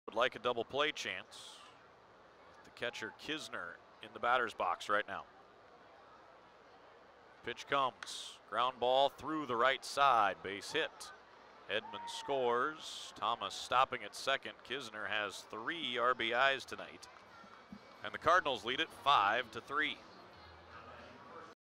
Knizner drives in third run of game
4/20/18: Springfield's Andrew Knizner grounds a ball through the right side for a single and his third RBI of the game.